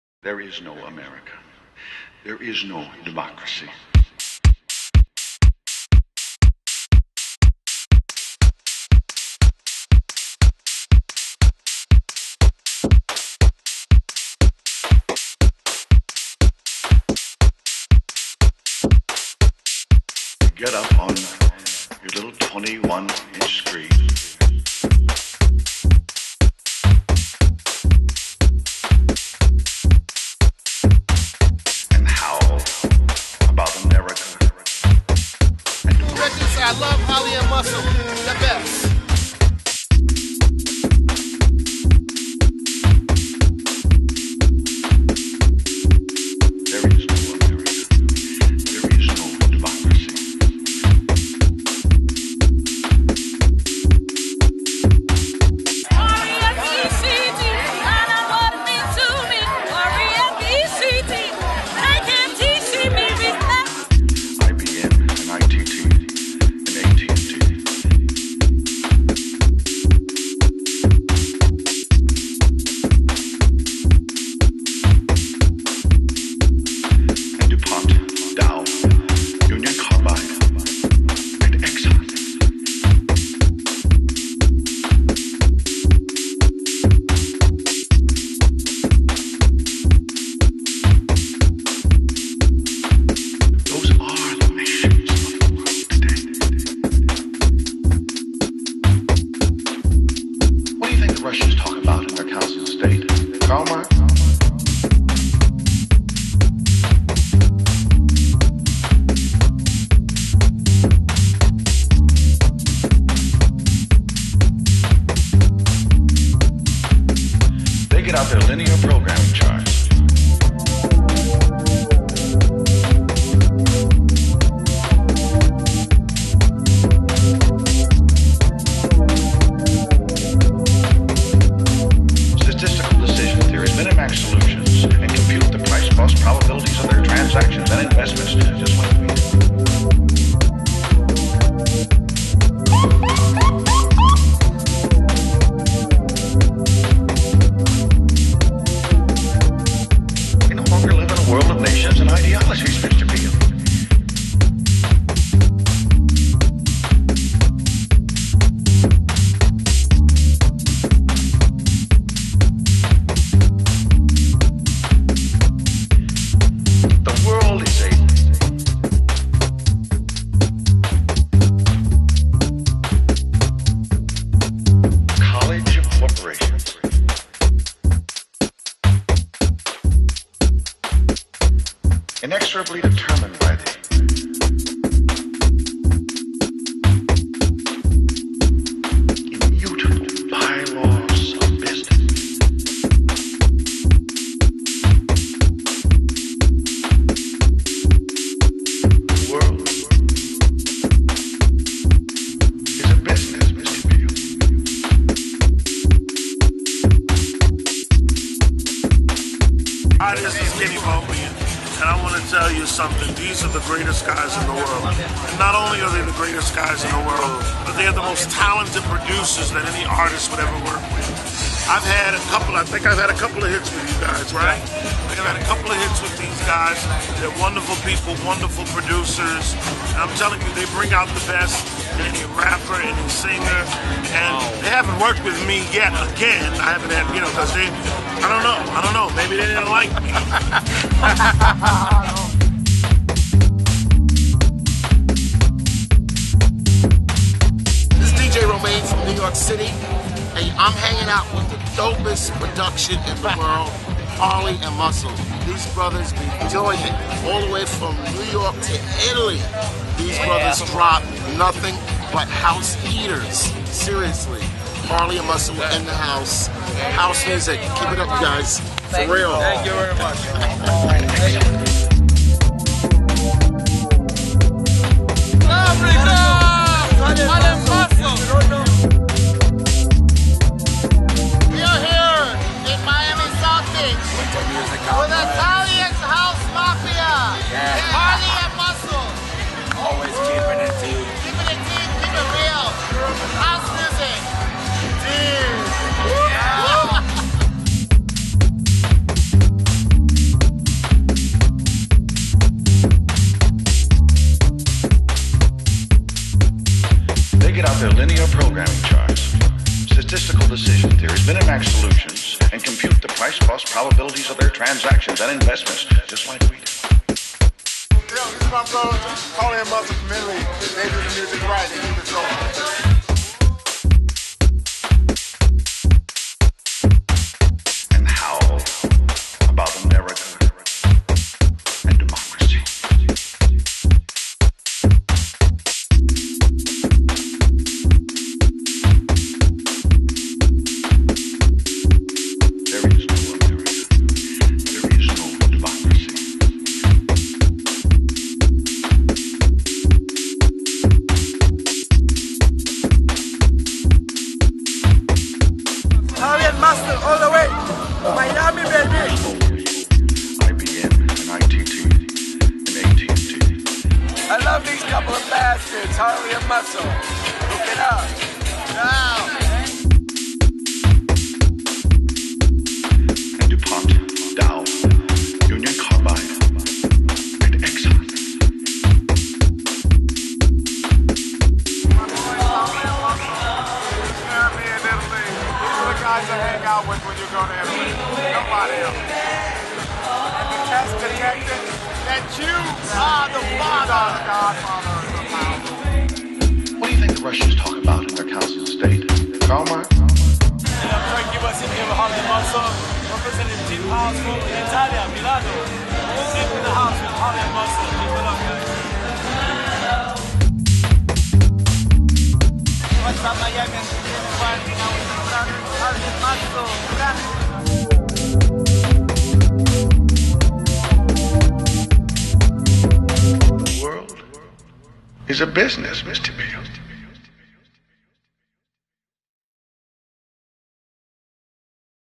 Harley&Muscle @ Winter Music Conference 2011
U can see the Italian Deep House dons, Harley&Muscle, playing @ the Winter Music Conference 2011 in Miami South Beach (Usa) and meeting some of their friends... Harley&Muscle rules!...